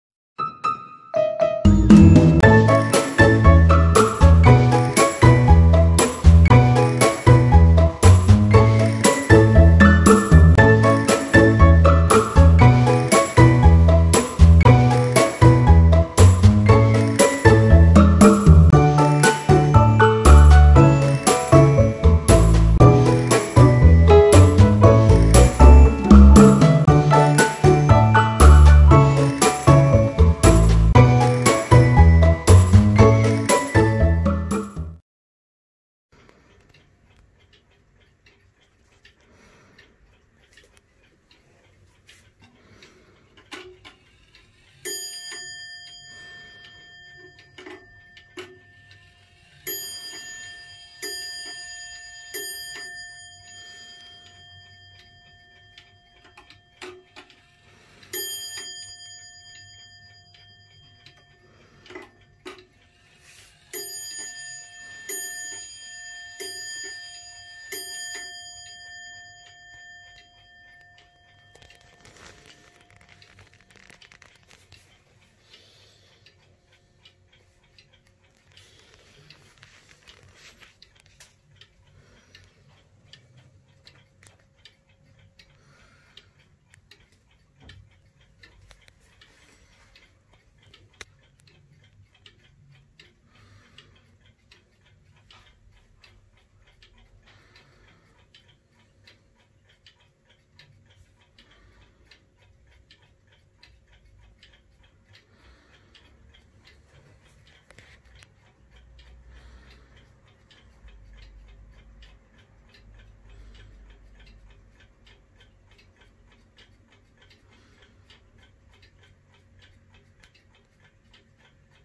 Comtoise Mini Wall Clock German SBS 8 Day Vintage Movement Bell Strike Vintage With Harp Pendulum
eBay